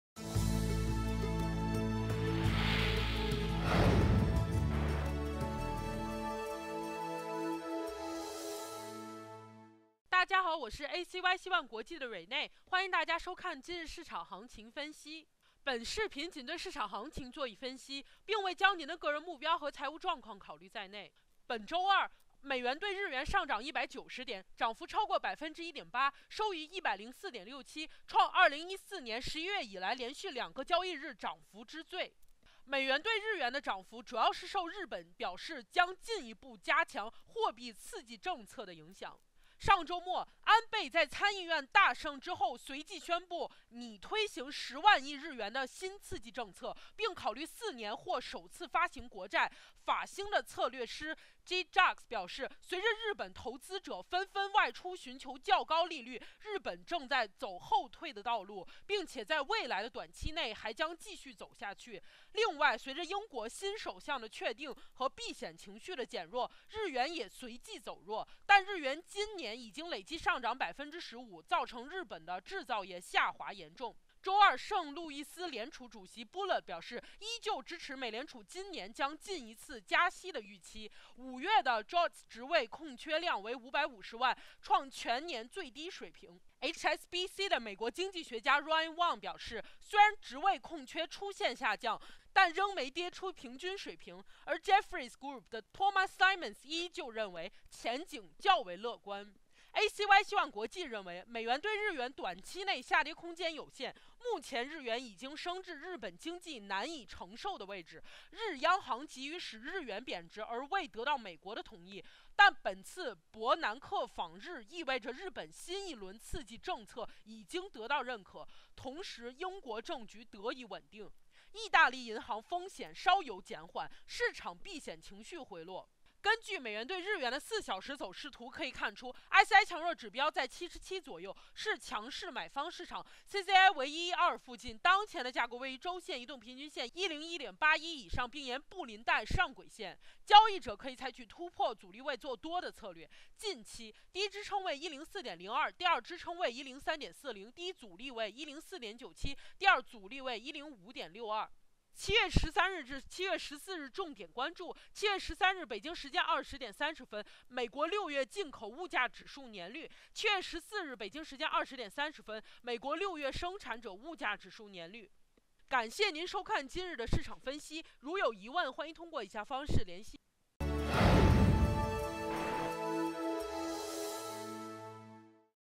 【ACY 稀万国际】日央行新刺激政策在即 日元两日暴跌
本周二（7月12日）美元/日元上涨190点，涨幅超过1.8%，收于104.67，创2014年11月以来连续两个交易日涨幅之最，如图一。美元/日元的涨幅主要是受日本表示将进一步加强货币刺激政策的影响。